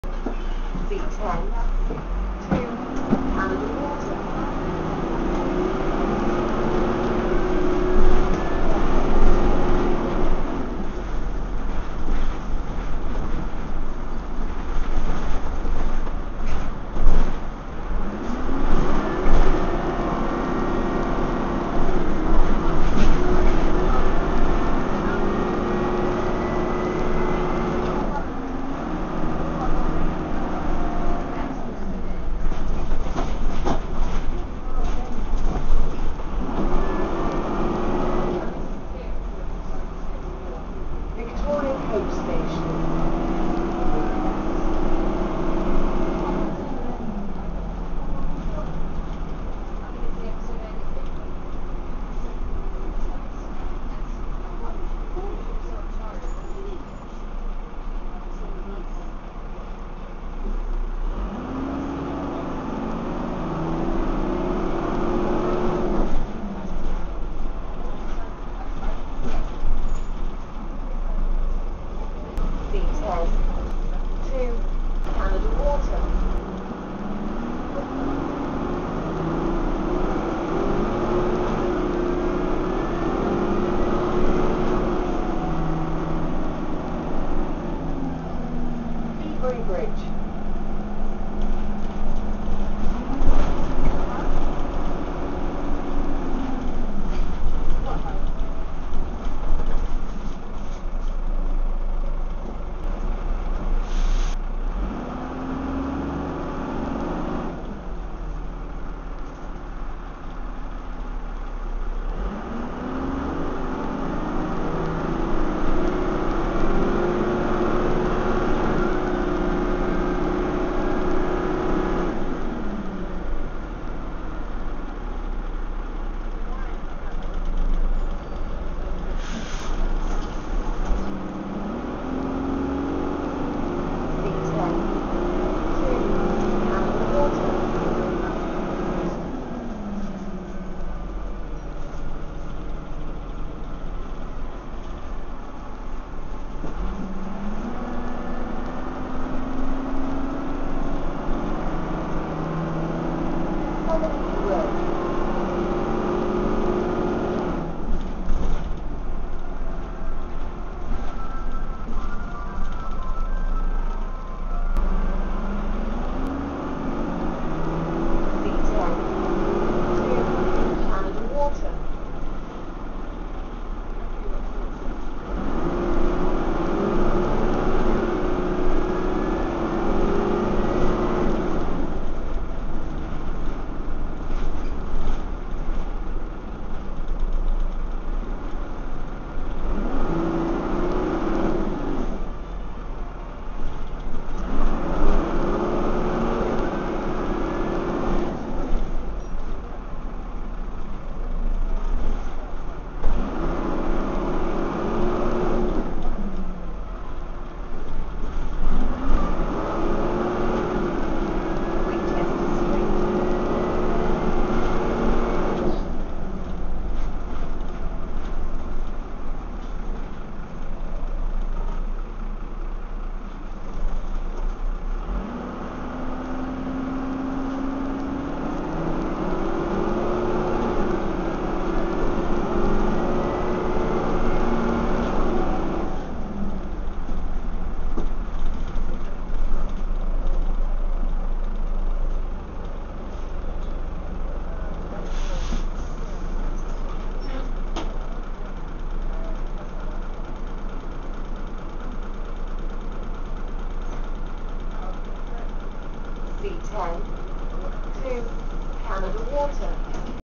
Journey On The C10 (LDP149,Y849TGH) Dennis Dart SLF 8.8m/Plaxton Pointer 2
LDP149 serving Route C10 in Victoria.
Route C10: Victoria - Canada Water.
Operated by Go Ahead London - New Cross (NX) Garage

Note: This Saturday (19th March) Route C10 will be handed over to Abellio London